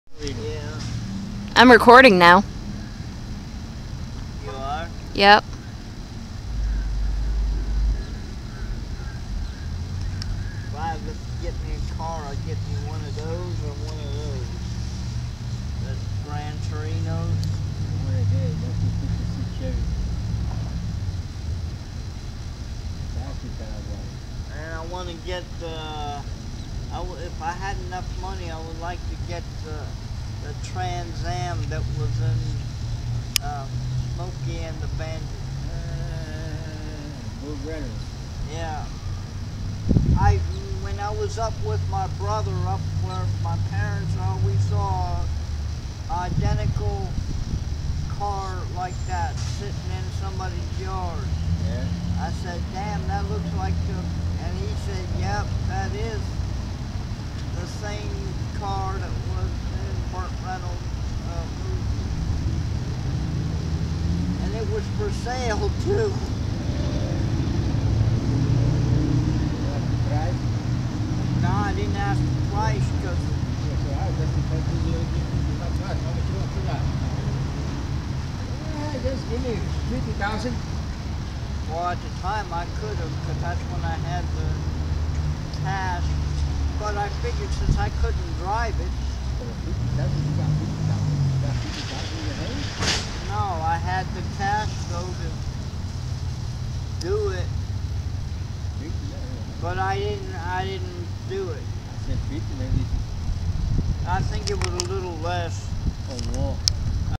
Spoonbill eating
Took this video Jan 2016. I love spoonbills and had been really happy to catch this video of it. I was standing on a bridge and my neighbors were talking to each other lol I tried to hint about recording so they'd quiet down, but oh well lol